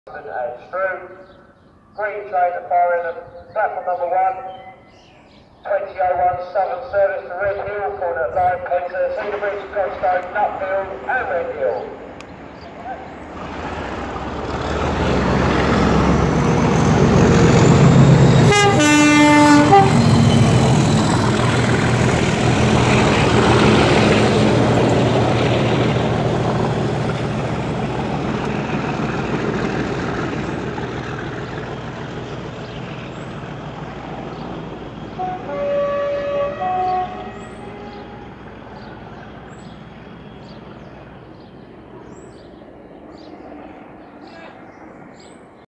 Large Logo 37407 Erupts through Tonbridge hauling DRS 88007 from Ramsgate 8/6/19
The first ever visit of a Class 88 to Kent coming to a close...